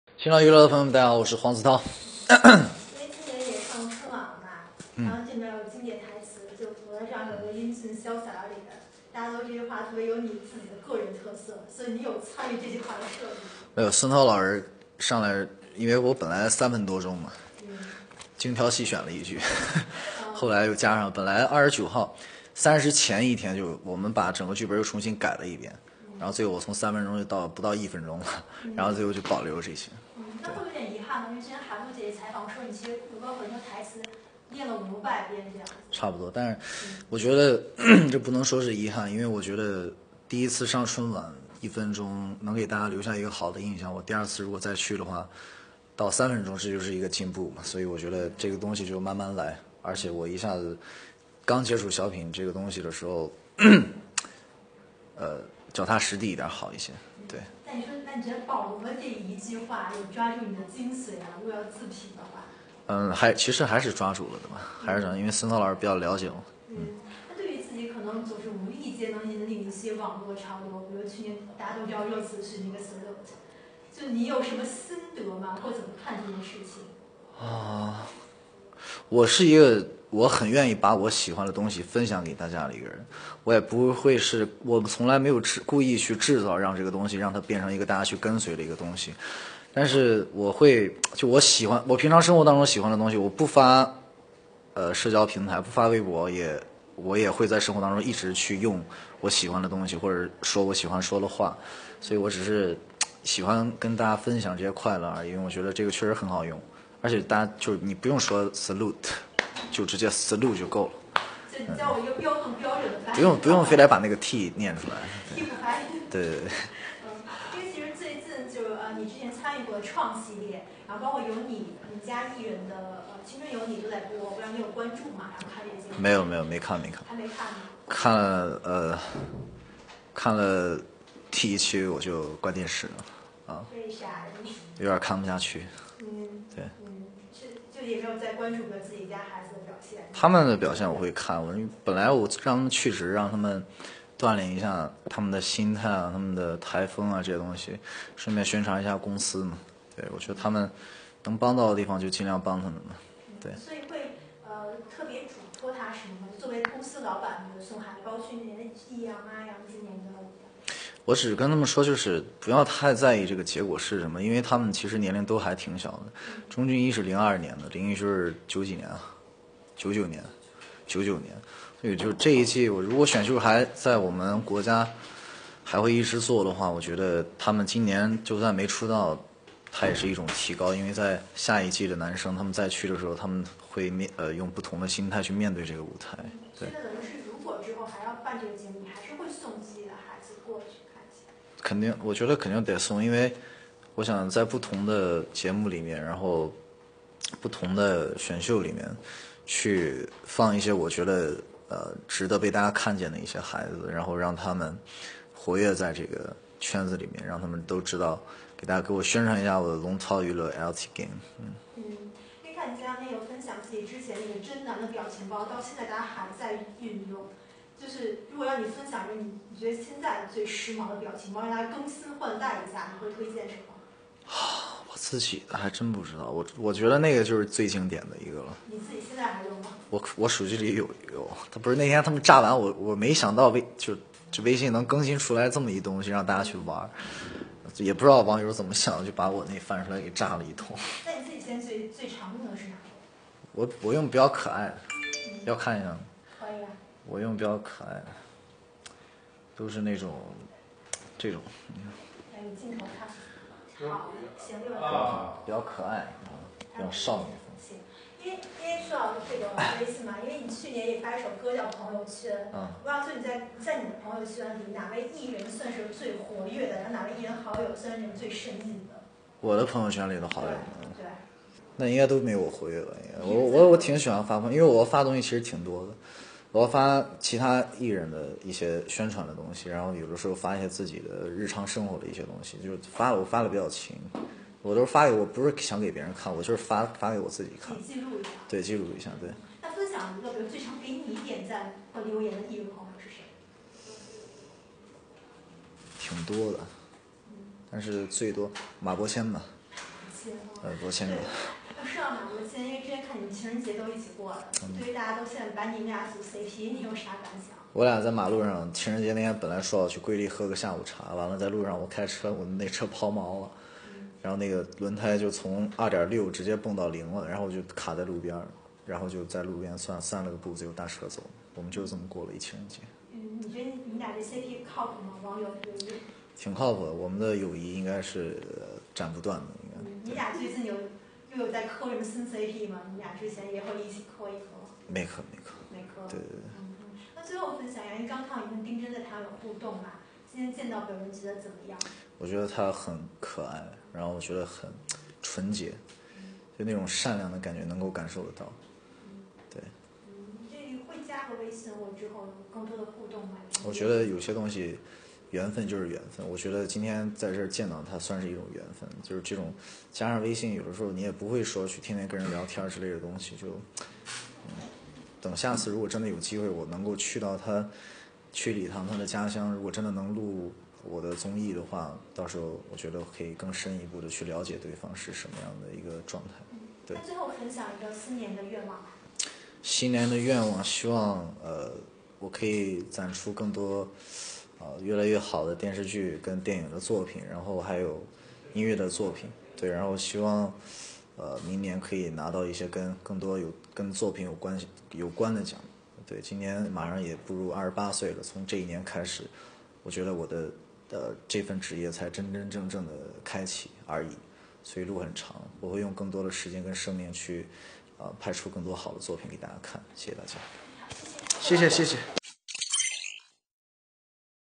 [黄子韬] 2020微博之夜专访
【2020#微博之夜#专访】黄子韬salute的t不发音#黄子韬 看了第一期我就关电视了# 对于自己总能引领网络潮流，微博年度热度人物@YKYBtao-黄子韬 表示自己不是想故意制造什么，只是乐于把日常喜欢的东西分享给大家。他还特意矫正记者英文发音：“你不用说Salute，你就直接Salu（音）就够了，T不用发音。”有关注《青3》《创4》吗？韬韬耿直表示：“看了第一期我就关了视频，有点看不下去。